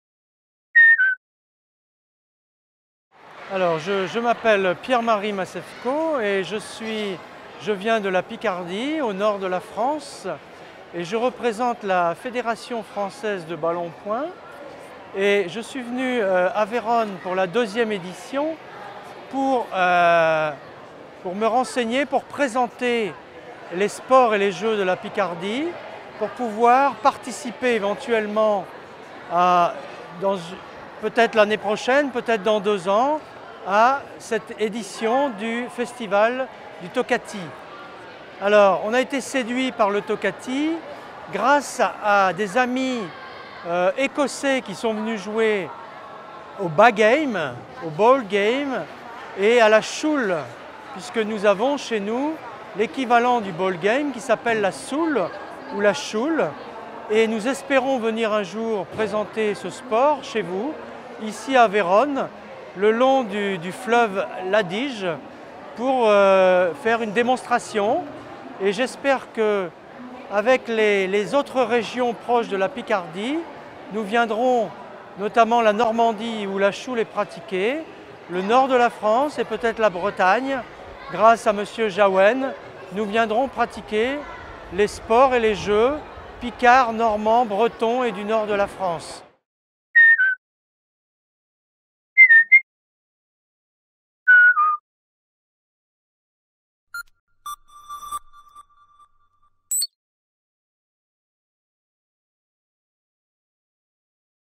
Pierre-Marie Macewko
Pierre-Marie Macewko, Fédération Française de Ballon au Poing - Federazione Francese di Palla Pugno è ospite al Tocatì come delegato della regione della Piccardia, Francia. Ogni anno il Festival riceve numerosi delegati di paesi europei ed extra europei che scambiano conoscenze e saperi, in qualità di esperti e giocatori.